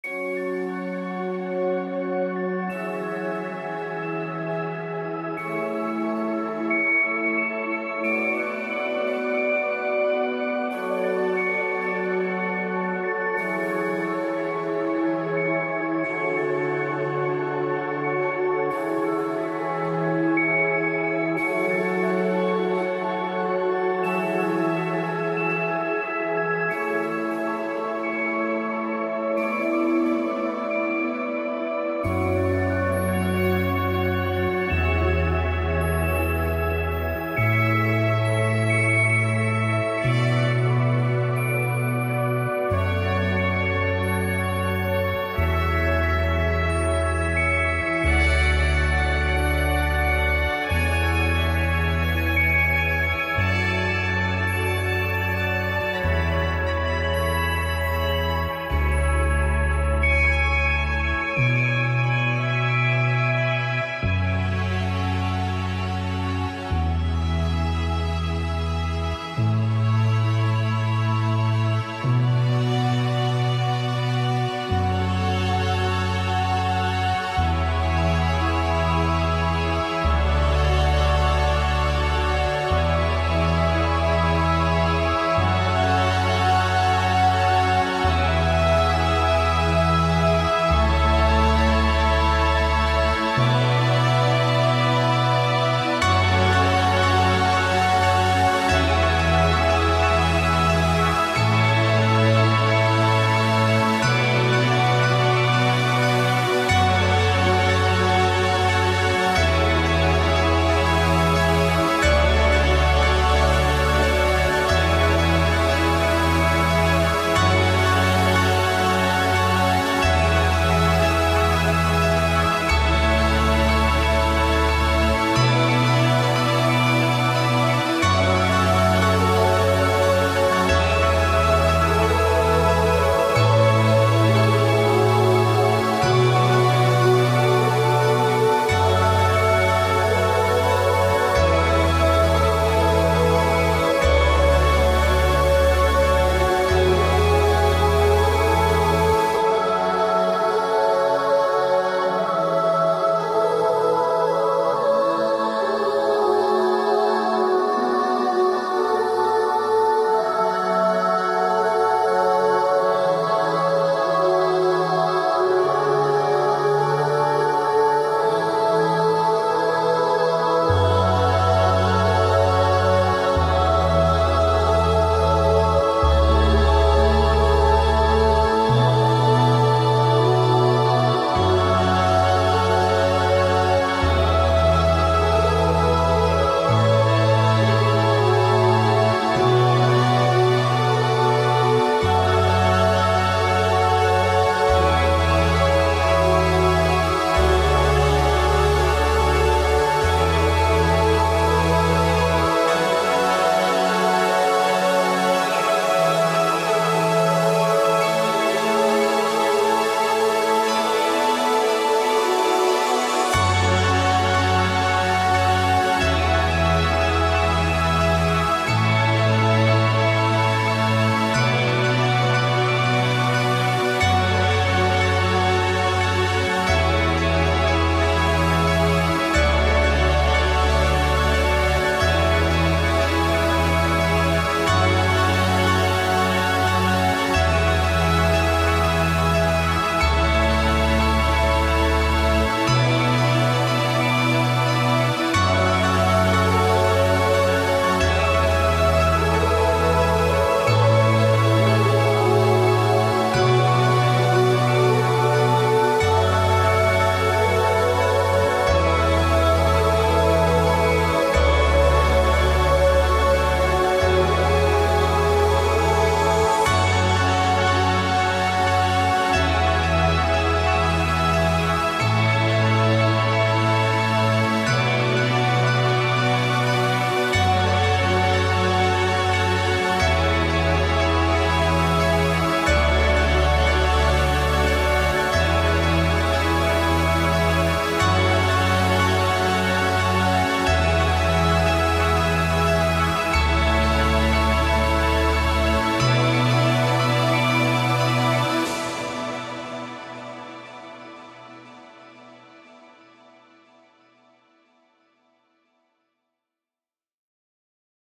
Dreaming Of Middle-Earth (AI Animation) - Lord Of The Rings AI Render
The Lord Of The Rings AI animation created by Peter Bergmann.